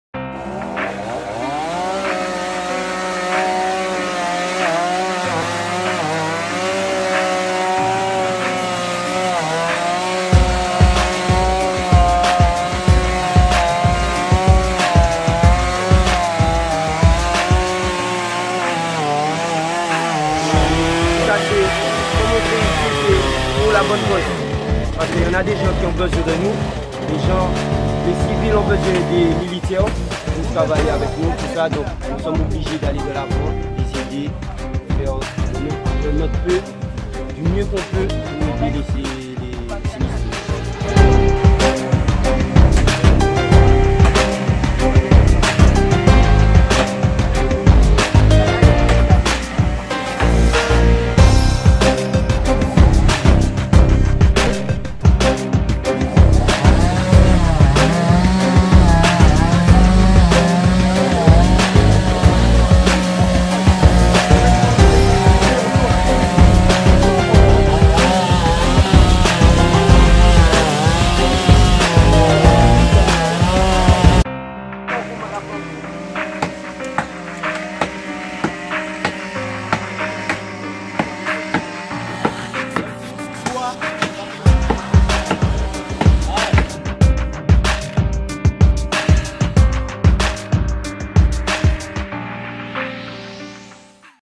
ouragan Maria : Une famille enclavée en Martinique - Identité Radio
travaux de désenclavement d'une famille en Martinique, après le passage de l'ouragan Maria le 18 septembre 2017